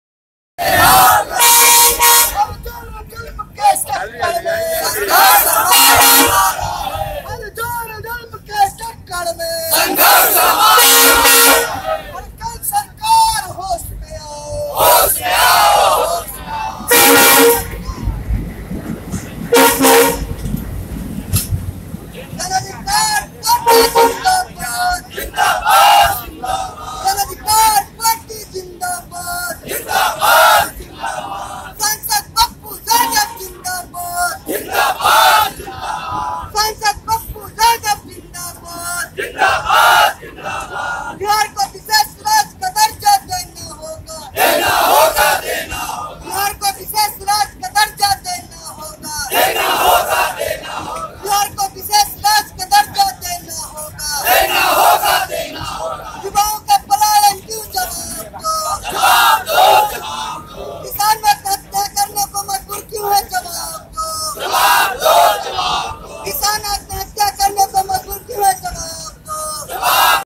Dr munna Khan Darbhanga
बिहार को विशेष राज्य का दर्जा दिलाने के लिए जन अधिकार पार्टी(लो.) के कार्यकर्ताओं ने पूरे बिहार में रेल- चक्का जाम करने का काम किया है, और दरभंगा_रेल्वे_स्टेशन बिहार संपर्क क्रांति को रोका गया !!